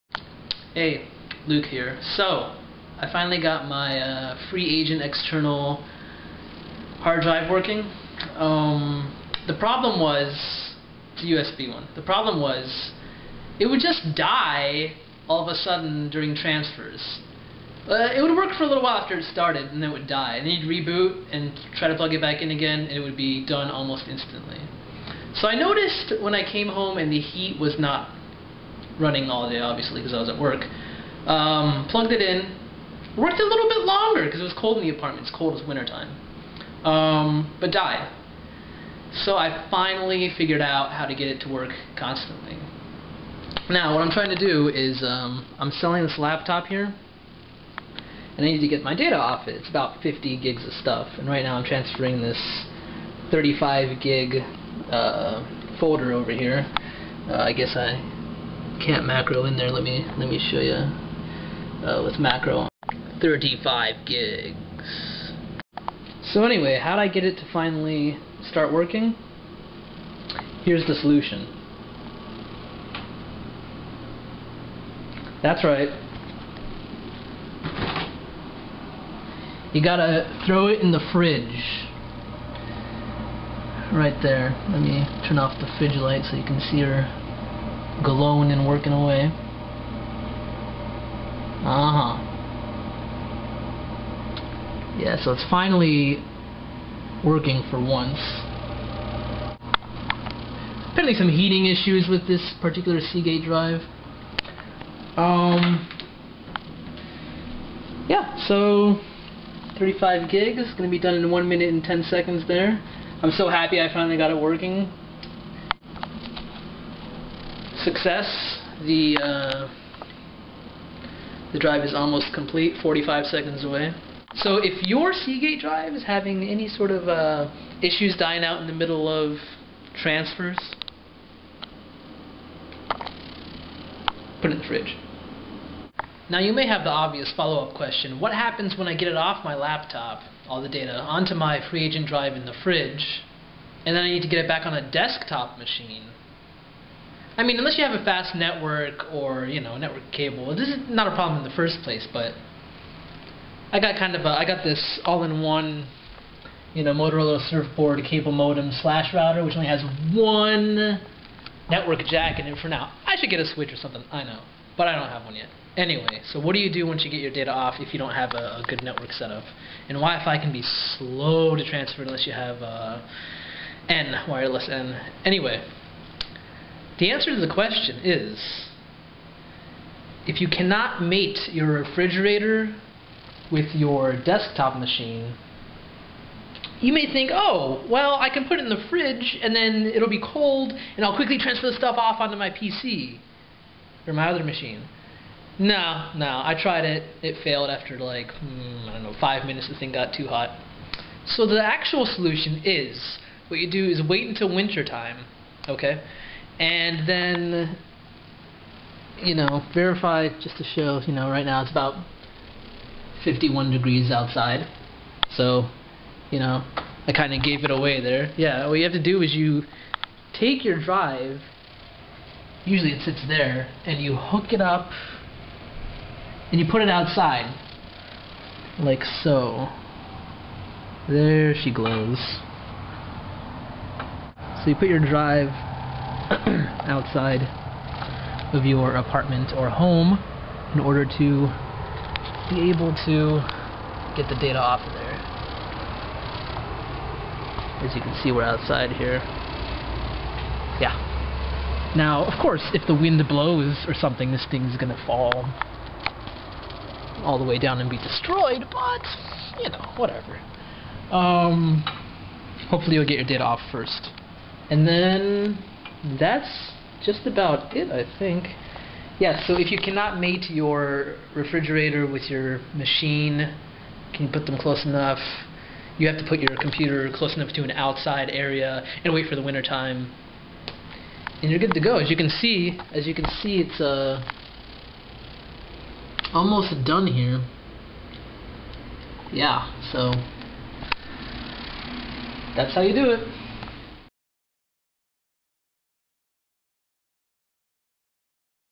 FreeAgent Solution
How to get a failing Seagate FreeAgent drive to work.

This particular 500gb FreeAgent USB drive stopped working after a couple months, and then would only transfer files for a short period after being powered on before giving errors and becoming unreadable.